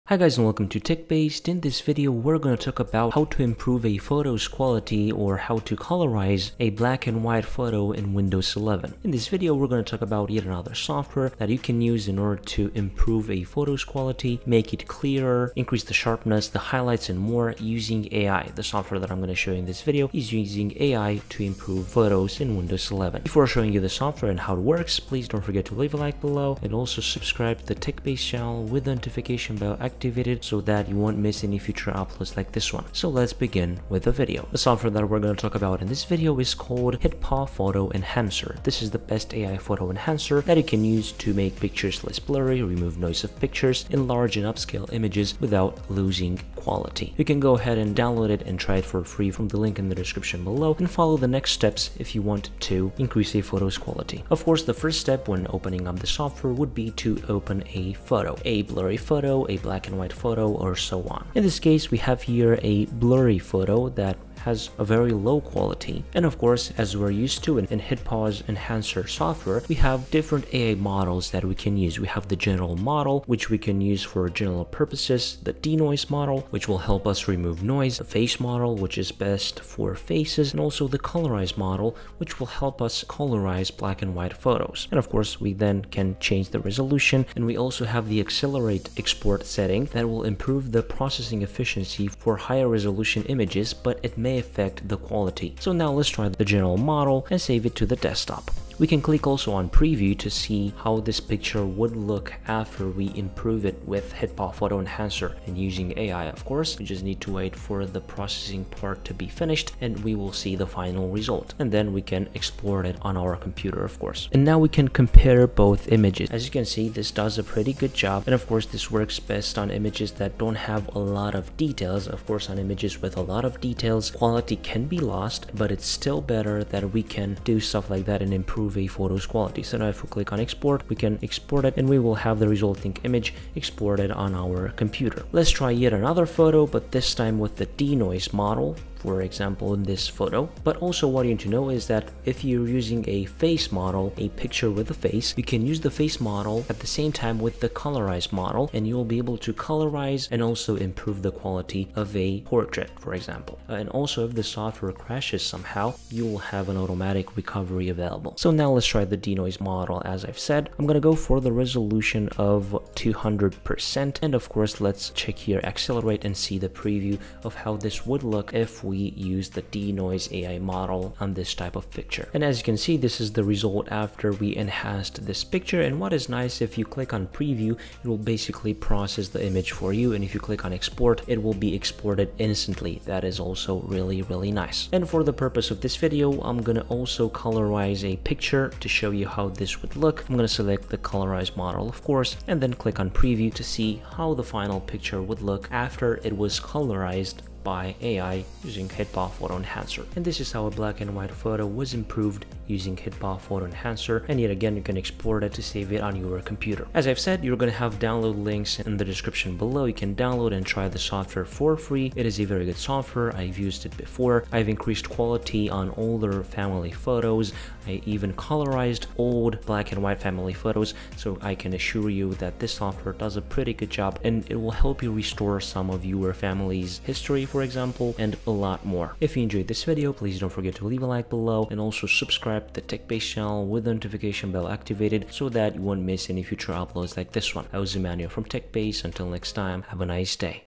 Best Software to Enhance Photo Quality in Windows 11
HitPaw Photo Enhancer powered by AI can automatically enhance image quality and resolution, and make pictures less blurry 100% with one click. This Pro-level photo enhancement tool fixes unblur photos perfectly and 4 AI models (General Model, Denoise Model, Face Model, Colorize Model) are available to enhance photo quality on any occasion.